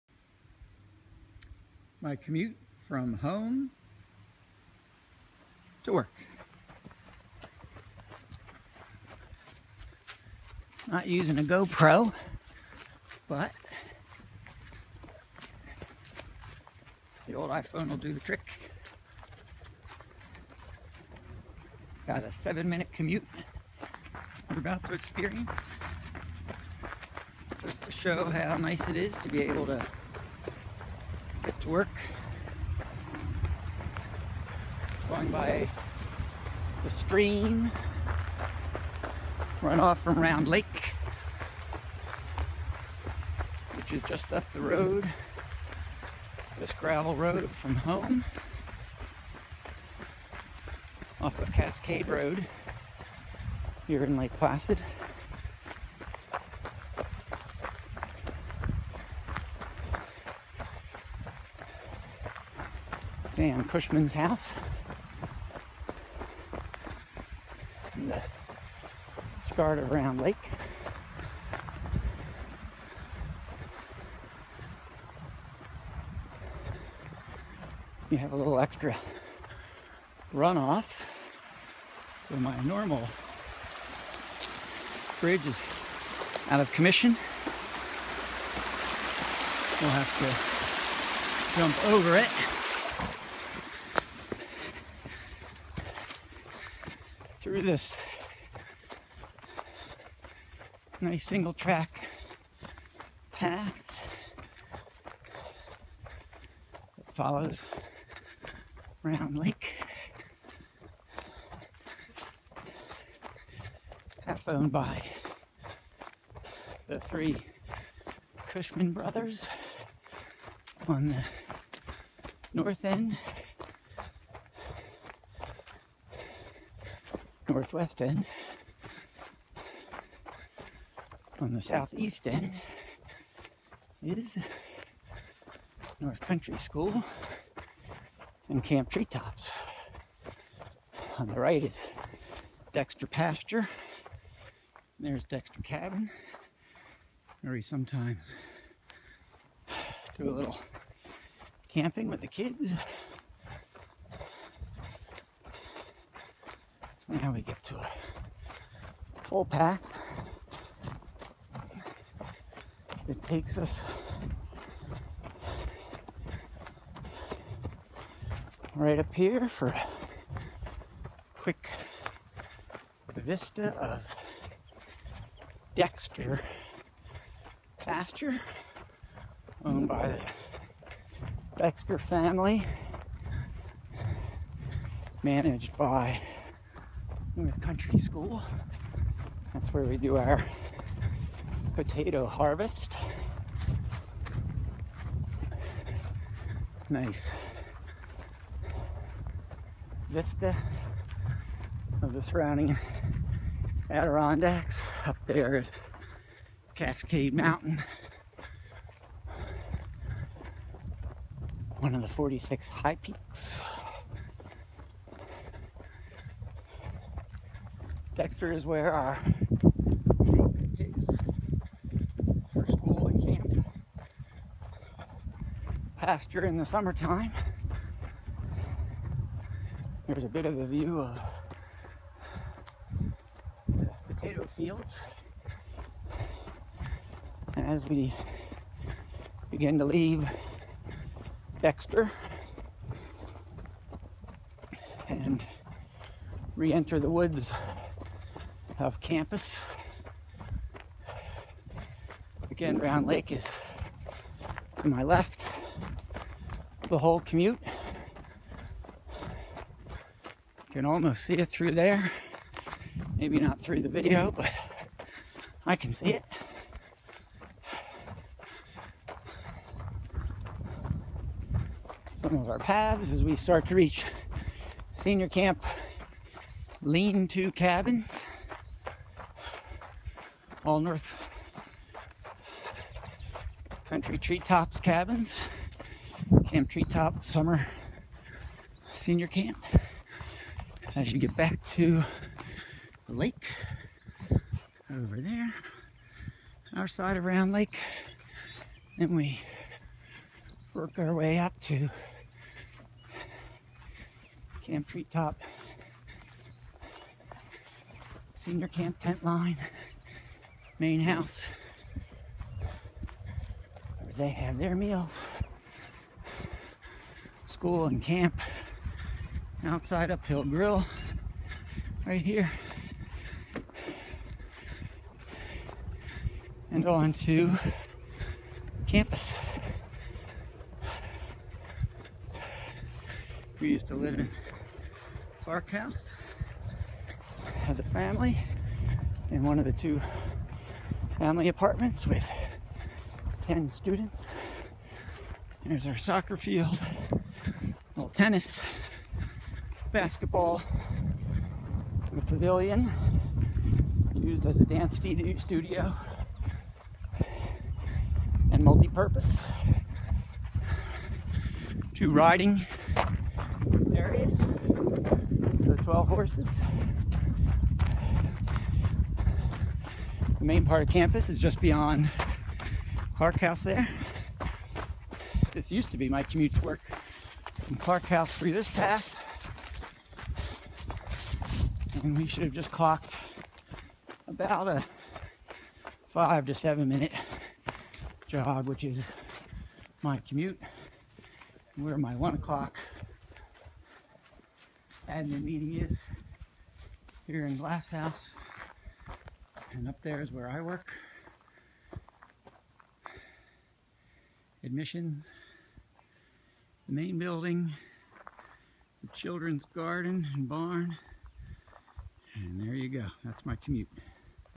David Damico commute to work by foot
Enjoy a jogging commute to North Country School and Camp Treetops